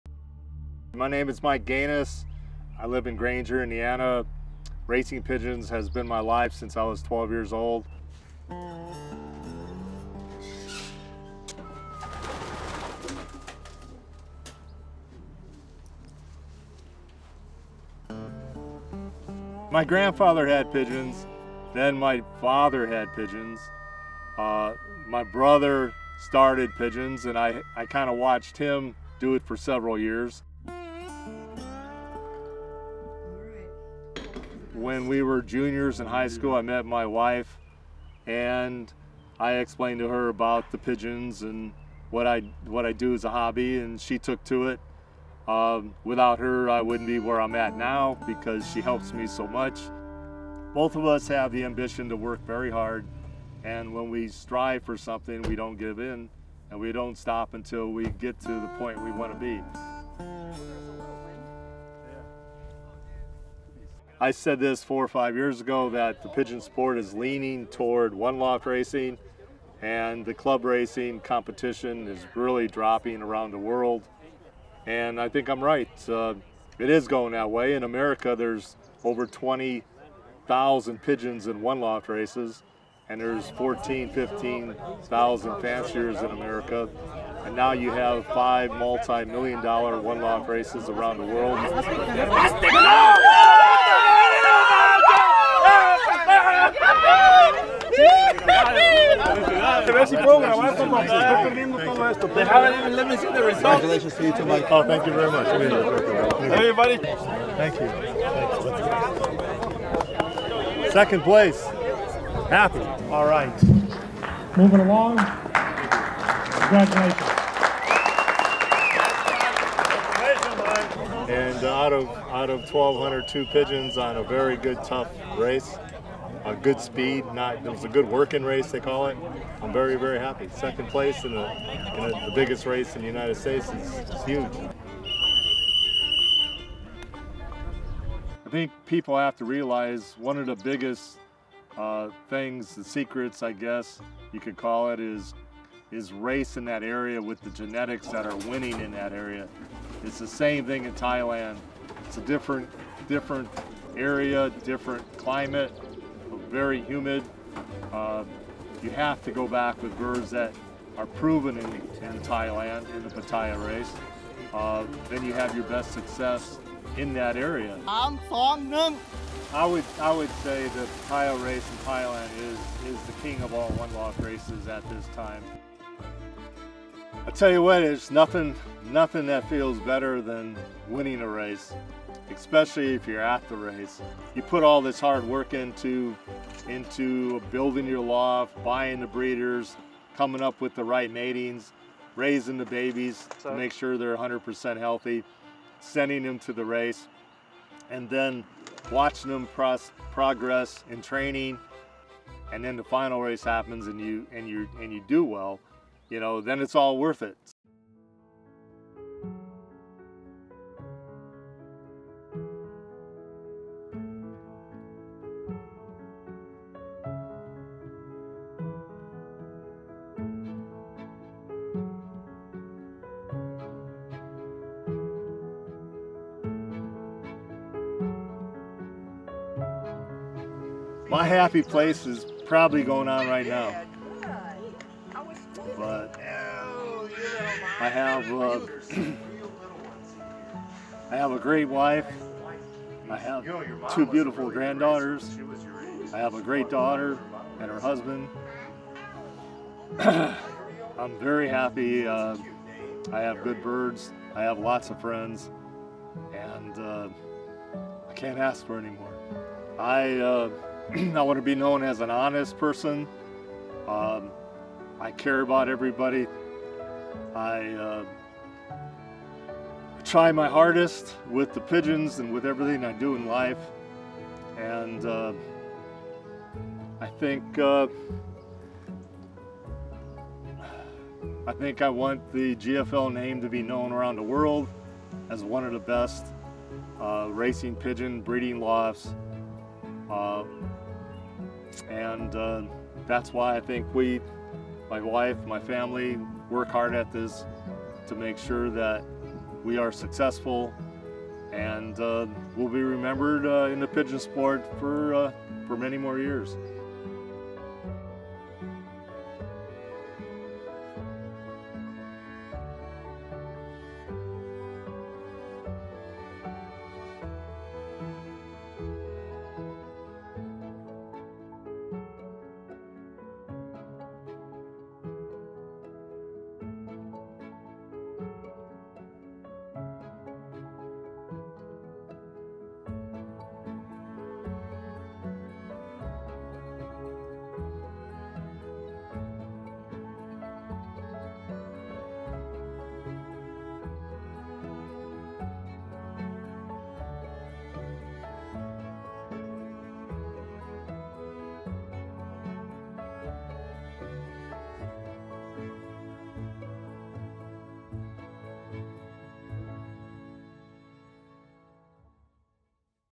The secret of Mike Ganus: Winners Grow New Winners
Follow the inspiring story of Mike Ganus, a racing pigeon enthusiast from Granger, Indiana, whose love for the sport was passed down from his grandfather and father. Since the age of twelve, Mike has dedicated his life to raising and training racing pigeons. With the help of his supportive wife, he has built a successful racing pigeon breeding loft and has achieved great success in one loft races around the world. Join us as we learn about the dedication, hard work, and passion required to be a top pigeon racer, and witness the thrill of victory as Mike celebrates his latest racing accomplishments.